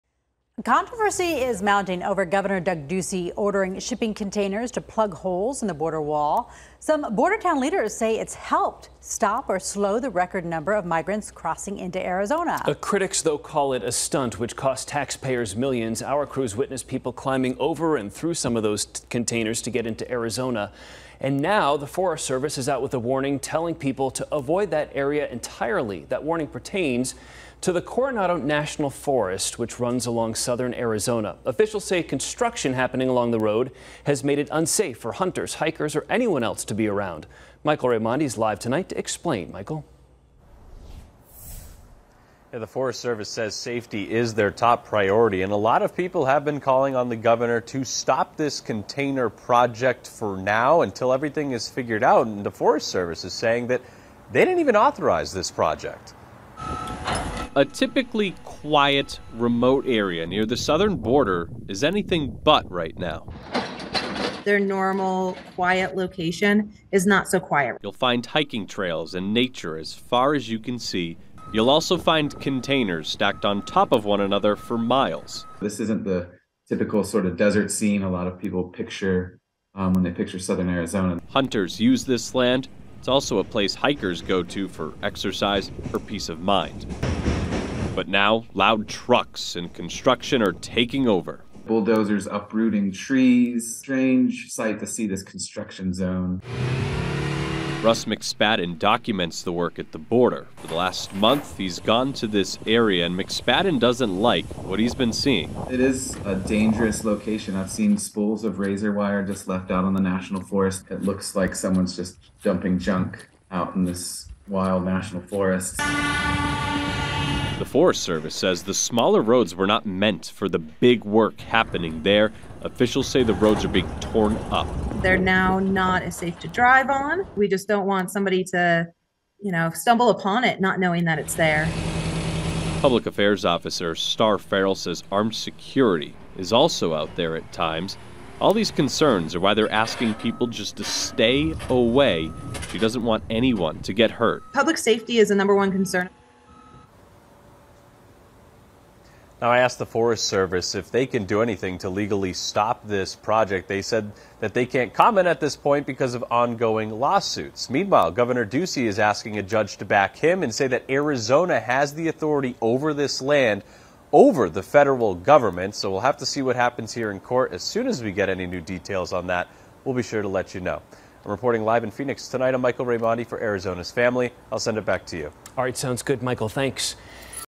Forest Service warning people to avoid shipping containers at the border
The Forest Service is calling the state’s project unauthorized and says related heavy equipment and the presence of armed securitymay create safety hazards.